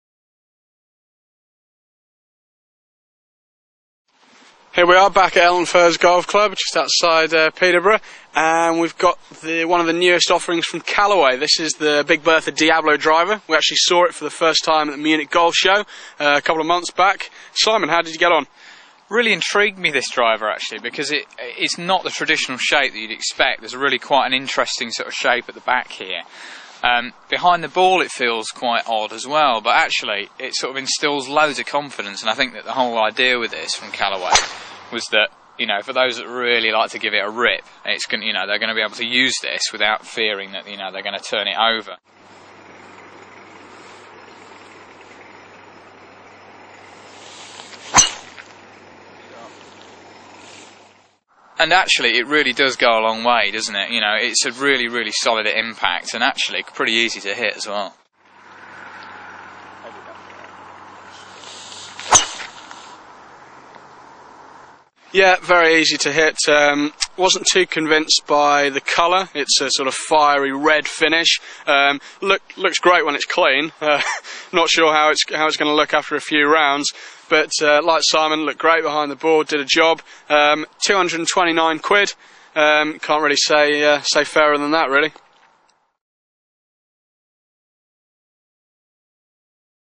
Callaway_Big_Bertha_Diablo_Driver_Review_Video_golfclubstore.us
Callaway Big Bertha Diablo Draw Driver
Finally, a driver that combines wicked power and distance with the generous sweet spot Callaway Golf's Big Bertha is famous for. Edge Technology provides a high MOI (moment of inertia) and an optimized CG (center of gravity) location for this Diablo Draw Driver that produces long, penetrating ball flights with the desired shot shape. It also helps golfers release the club at impact with full power, especially when swinging aggressively. Hyperbolic Face Technology increases ball speed across the entire face of the club helping send balls further down the fairway, even on miss-hits.